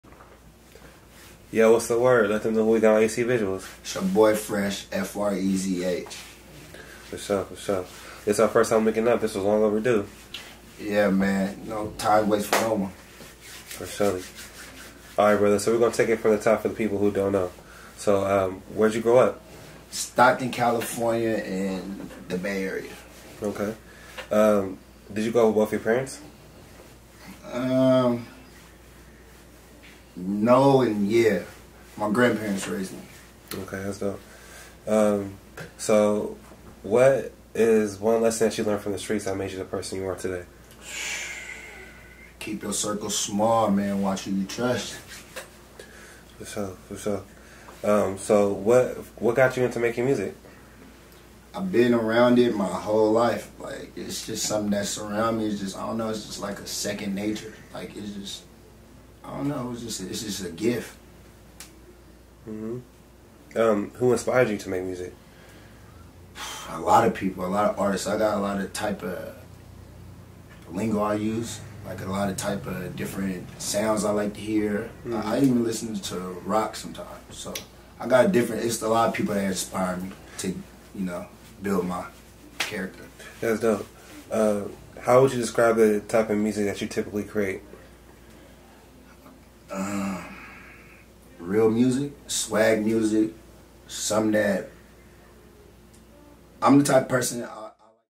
Frezh interview part 1
Fresh is from Stockton California local up and coming rapper from the south side

Follow on Instagram at @g6Fresh